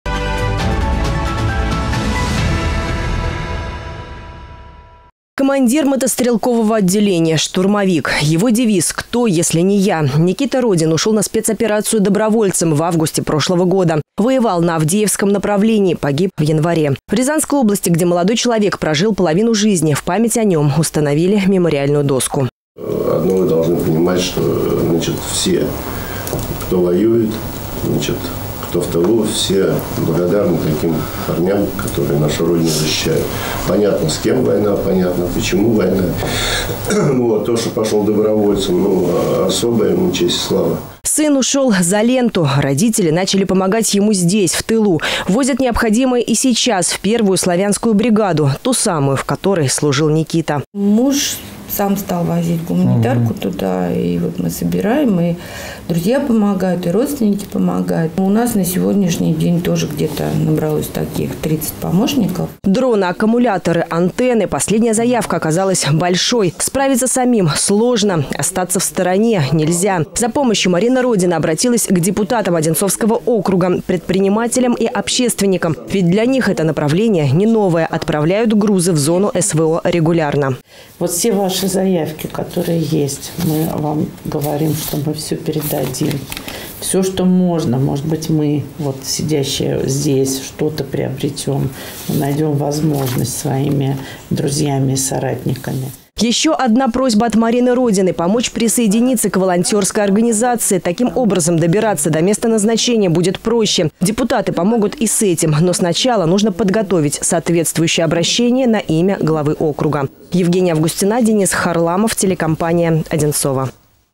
Помощь волонтерам
Не реже одного раза в месяц Марина Родина вместе с супругом возит в зону СВО гуманитарную помощь. Помогают подразделению, в котором служил их сын. Никита погиб в январе этого года. Последняя заявка оказалась большой. За содействием женщина обратилась к общественникам и окружным депутатам. В чем нуждаются волонтеры, узнала наша съемочная группа.